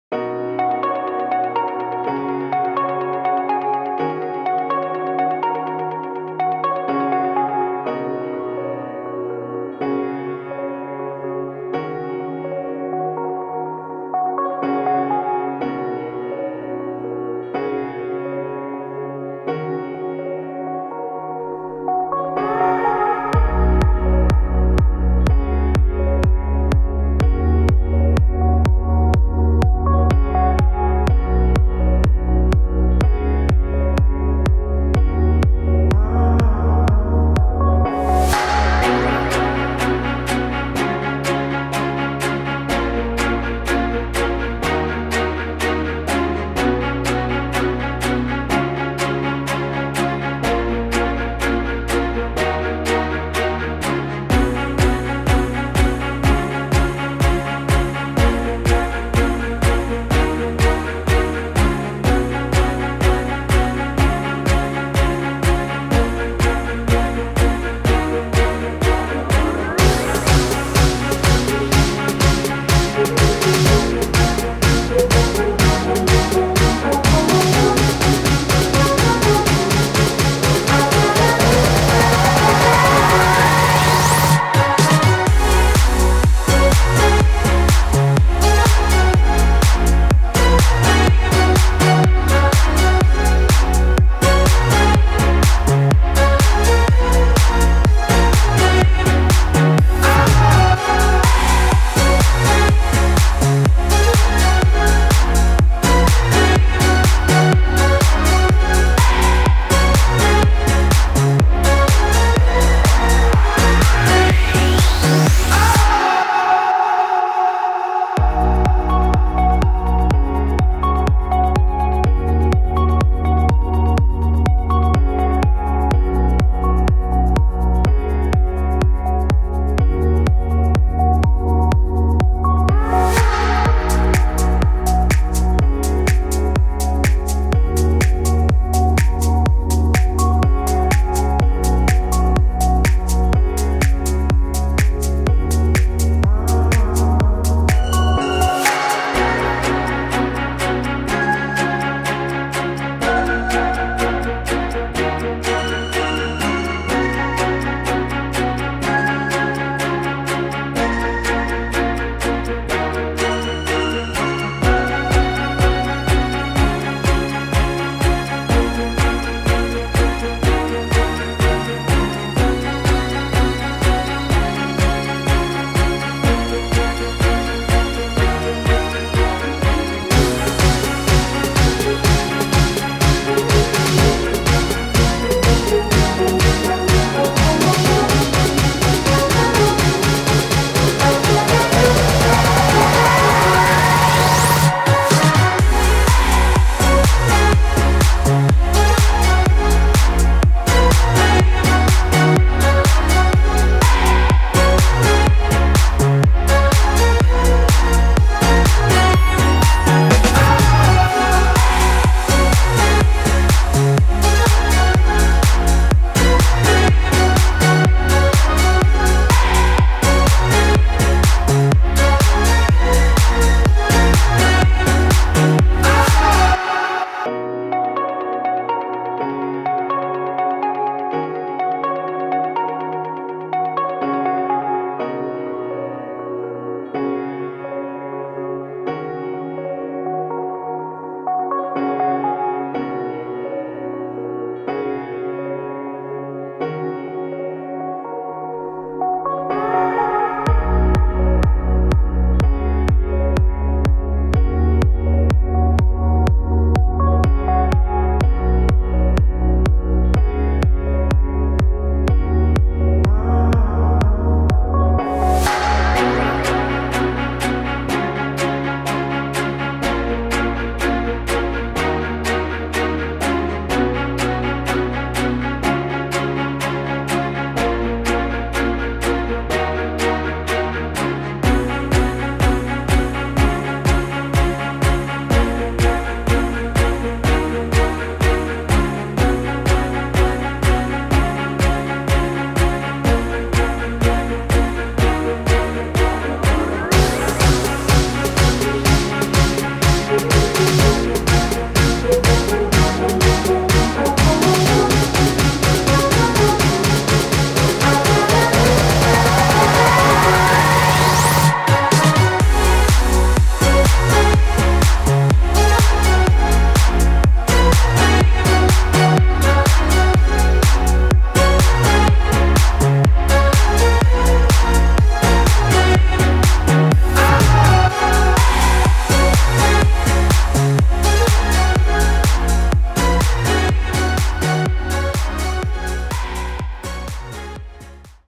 New BMW X7 Xdrive M60i 2023 | Expressive Front | Luxury BMW Model | Driving Scenes
Taking a look at the 2023 BMW X7 Xdrive M60i footage. In this video you can see the exterior & interior details.